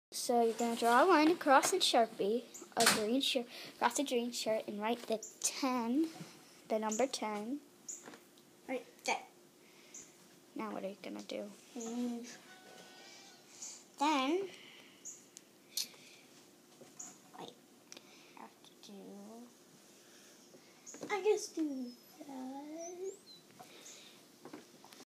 How to make a Cheap💰Homemade 🏠Ben 10 Shirt 👕
We are making a Ben 10 Shirt! To make it, you need two supplies: a green shirt and a black Sharpie/permanent marker.
Also, kids, ask an adult before making the Ben 10 shirt. They might not want you to use the shirt for putting permanent marker on it or they don't want you to use a Sharpie.
We think it is better if you have a parent or adult guardian help you do this craft. Even though you might think you're old enough to do it, all parents have different rules. Besides, they might have great tips on the best way to do it. Maybe it would be even better than ours!
We will put another video on YouTube so you can see the results of the Ben 10 shirt craft we did. We also hope your Ben 10 shirt looks amazing and cool!